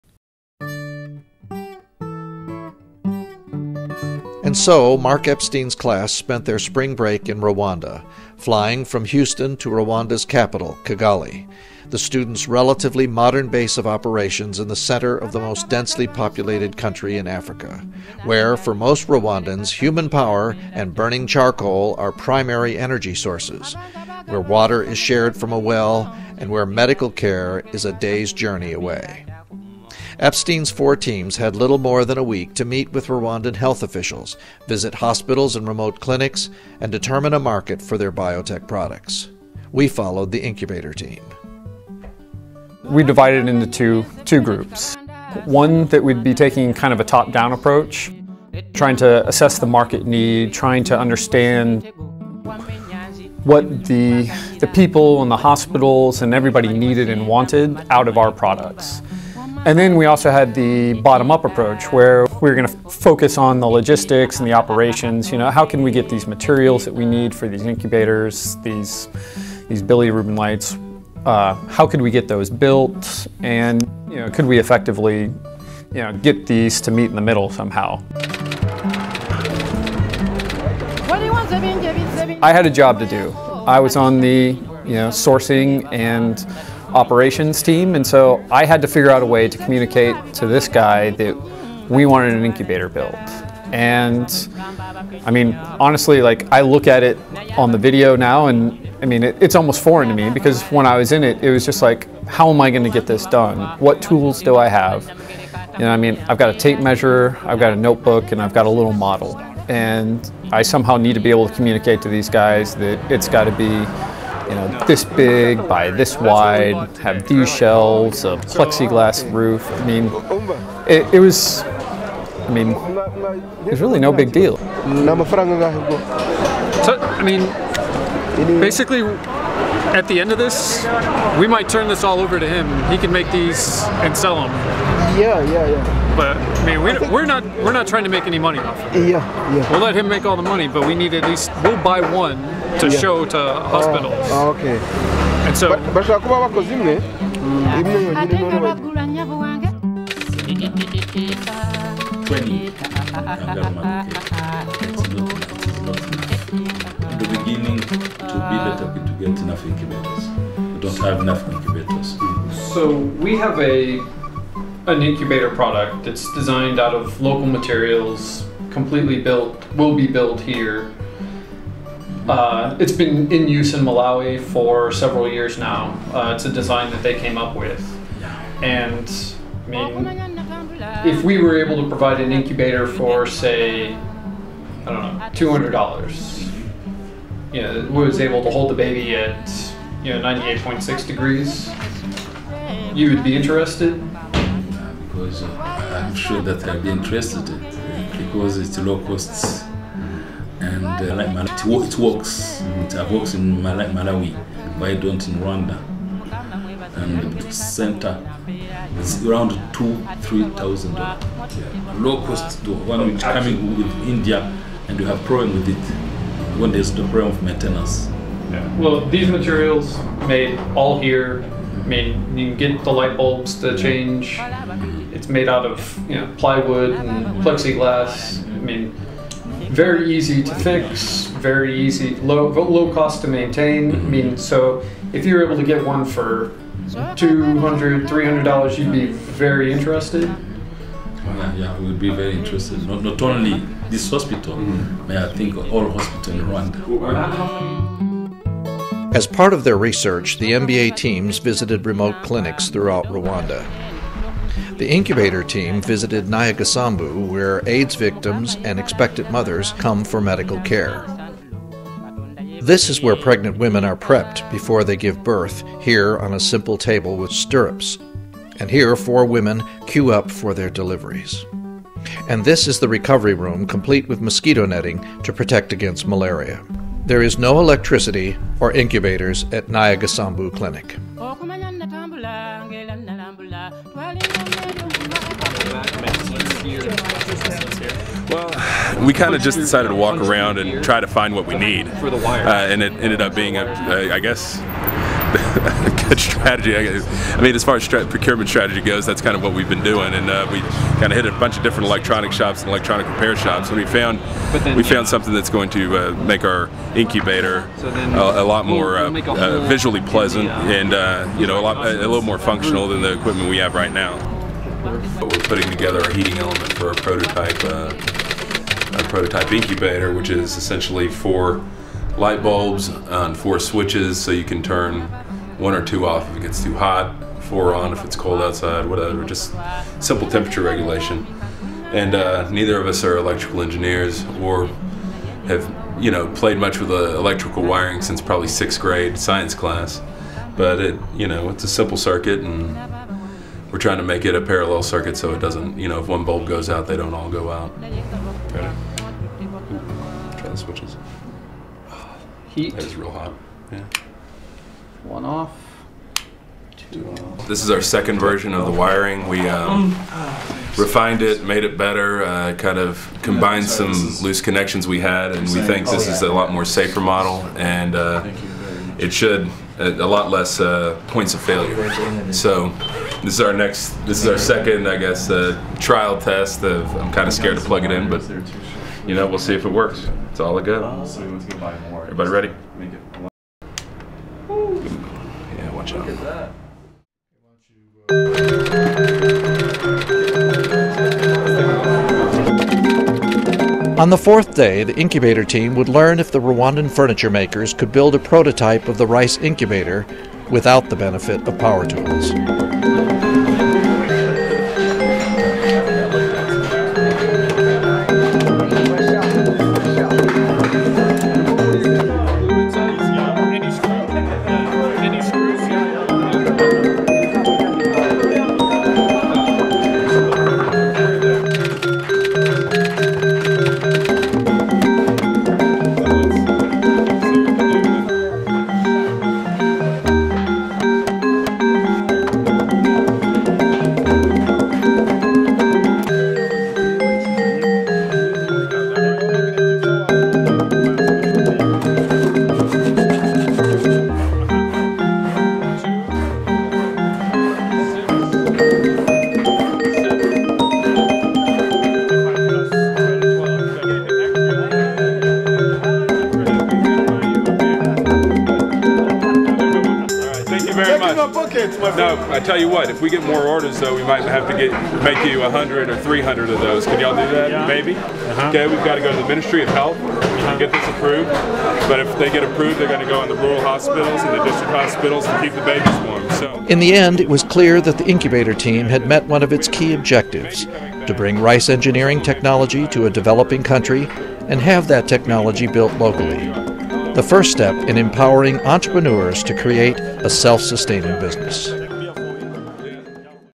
Project Rwanda -- Part II (Building an Incubator)
Part II -- 20 Rice MBAs took off on a 10,000 mile journey to Kigali, Rwanda -- the heart of Africa -- where they met up with their professor, Marc Epstein, to help Rwandan entrepreneurs launch businesses using products developed and tested by Rice engineering students.